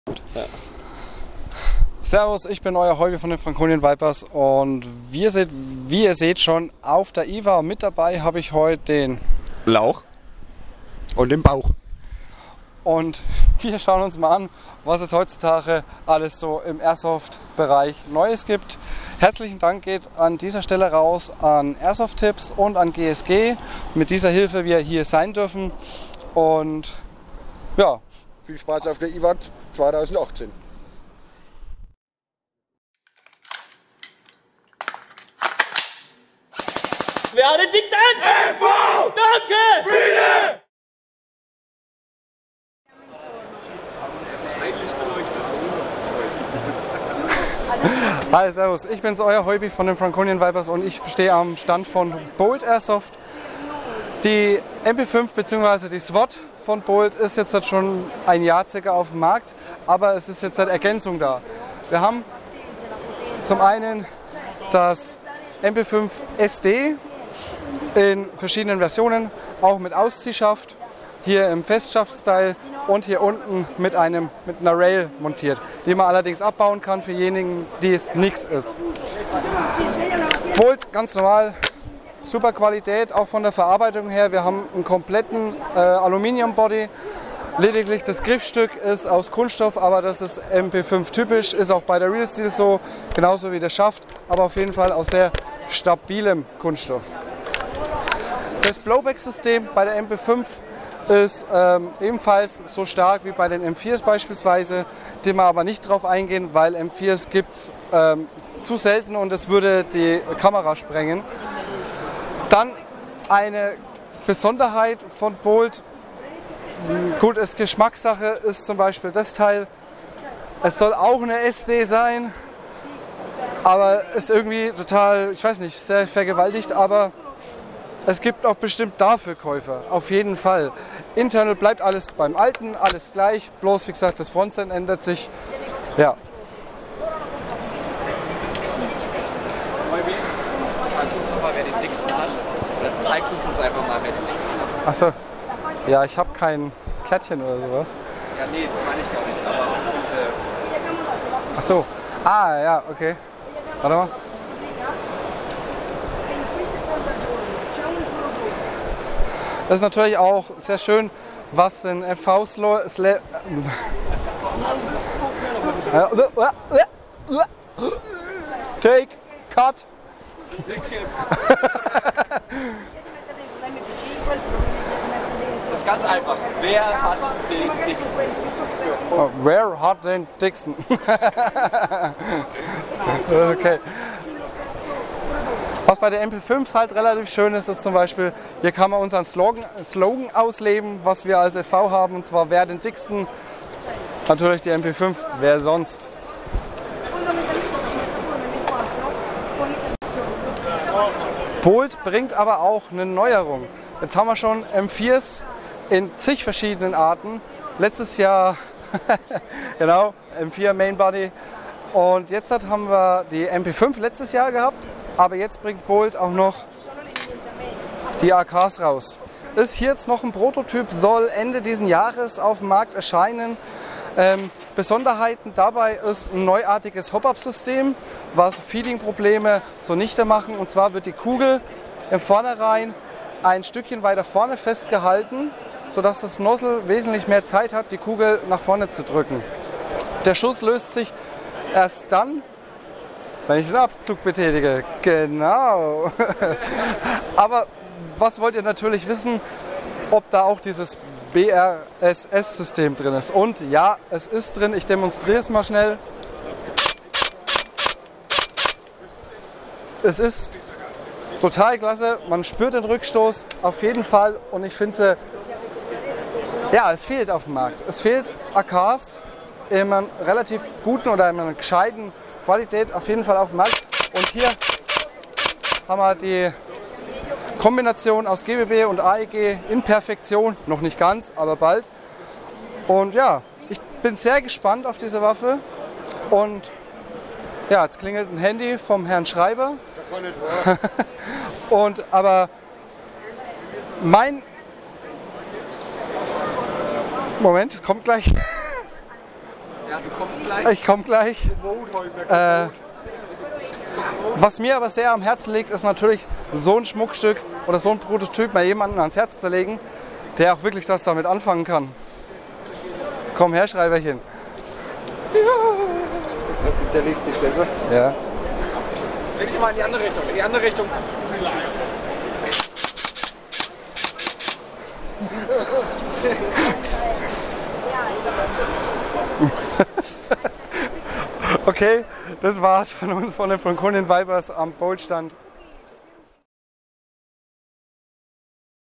IWA 2018 | Bolt Airsoft
Wir waren auch zu besuch bei Bolt Airsoft hier wurde die MP5 um zahlreiche Modelle erweitert.
Und man sieht einen Prototypen, die neue AK mit BRSS System.